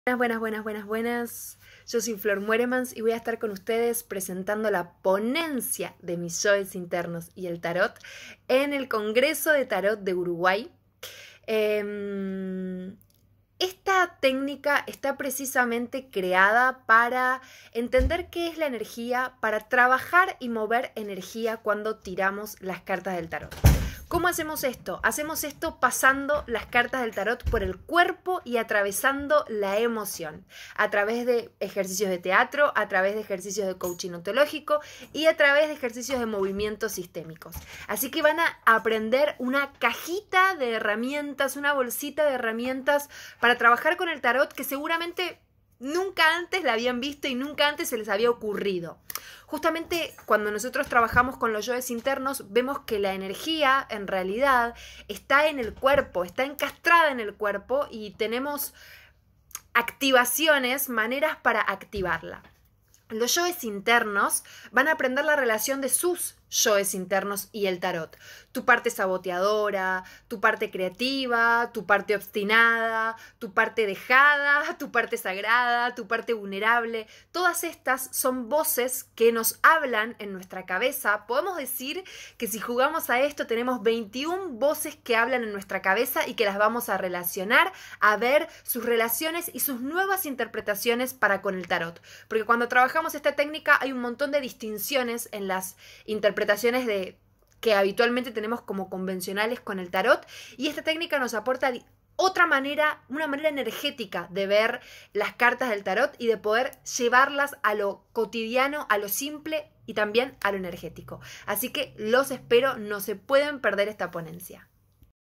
Congreso Tarot Uruguay 2021 - Florencia Moeremans - Video Presentación
Florencia nos presenta su ponencia "Mis yoes internos y el Tarot"